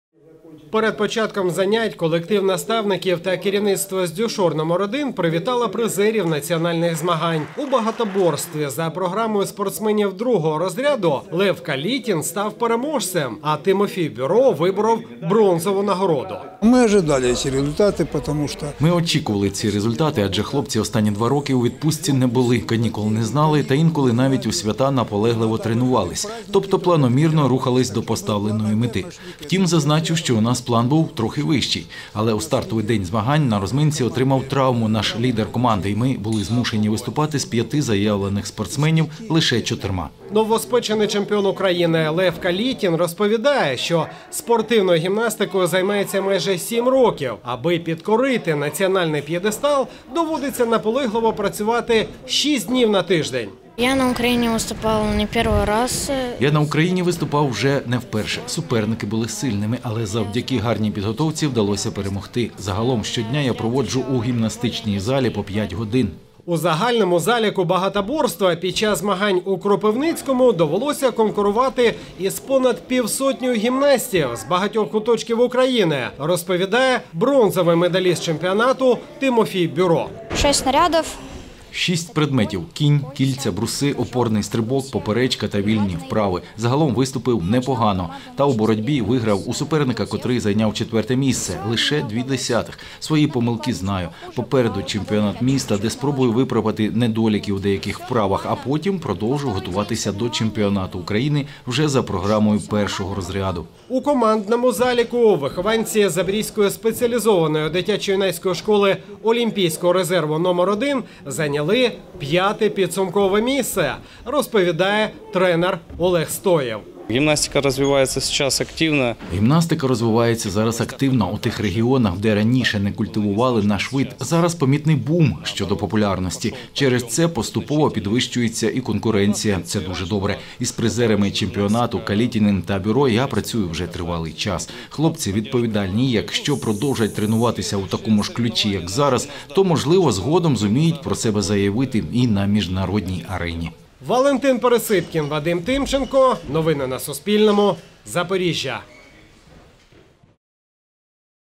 Запорізькі гімнасти стали призерами Чемпіонату України | Новини
Запорізькі гімнасти відзначились двома нагородами на чемпіонаті України серед юнаків. Журналісти Суспільного побували на тренуванні призерів національних змагань після їхнього повернення додому, аби дізнатись про подальші плани учнів місцевої спеціалізованої дитячо-юнацької школи олімпійського резерву.